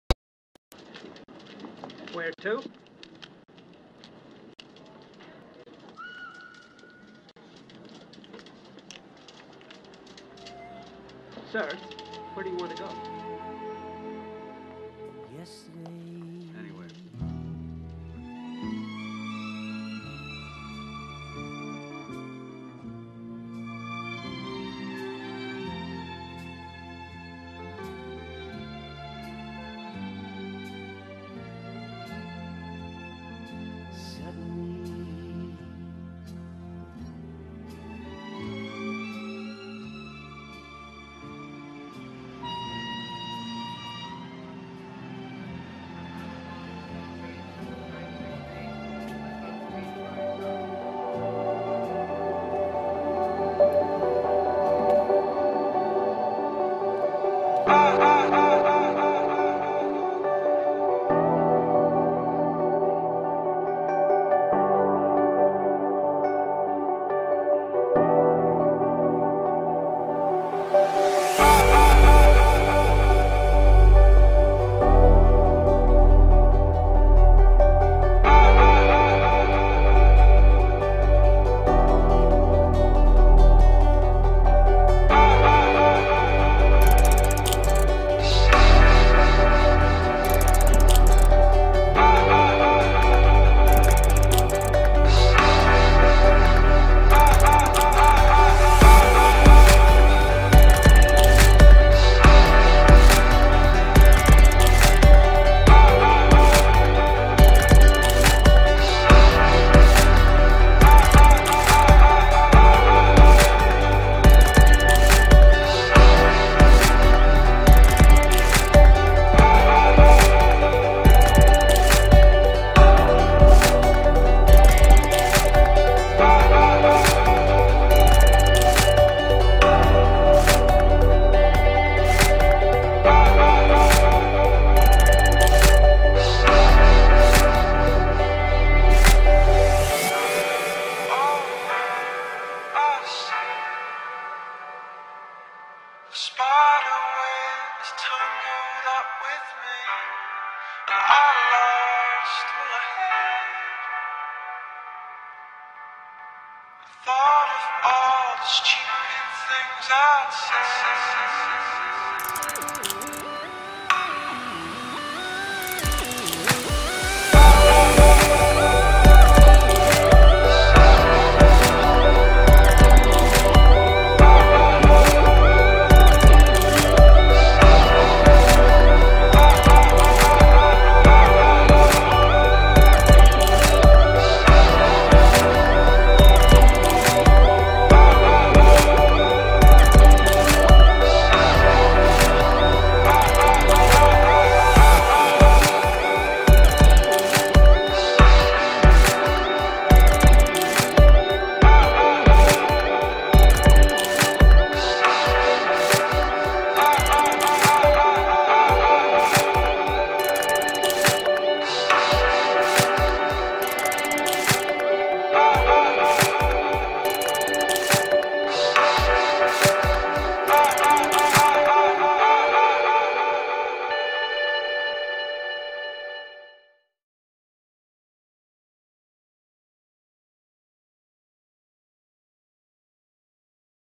I go to Aussie
Just a little video from my trip to see UFC 193 and some friends :)